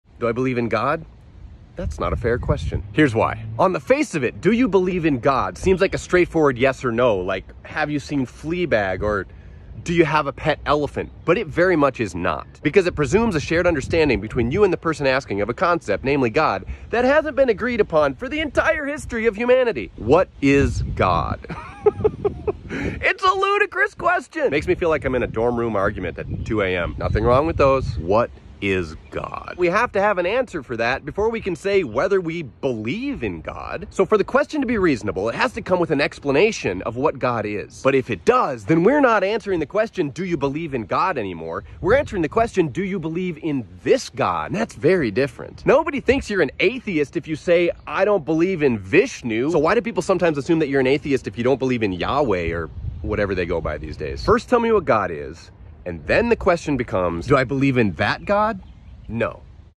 "Do you believe in god?" is not a fair question
Not believing in *your* god doesn’t make anyone an atheist. 🤷‍♀️

Transcription:

Do I believe in God? That's not a fair question. Here's why. On the face of it, do you believe in God seems like a straightforward yes or no, like have you seen Fleabag or do you have a pet elephant, but it very much is not, because it presumes a shared understanding between you and the person asking of a concept, namely God, that hasn't been agreed upon for the entire history of humanity.

What is God? It's a ludicrous question. Makes me feel like I'm in a dorm room argument at 2:00 AM. Nothing wrong with those. What is God? We have to have an answer for that before we can say whether we believe in God. So for the question to be reasonable, it has to come with an explanation of what God is. But if it does, then we're not answering the question, do you believe in God anymore, we're answering the question, do you believe in this God? And that's very different.

Nobody thinks you're an atheist if you say, "I don't believe in Vishnu." So why do people sometimes assume that you're an atheist if you don't believe in Yahweh, or whatever they go by these days? First tell me what God is, and then the question becomes, do I believe in that God? No.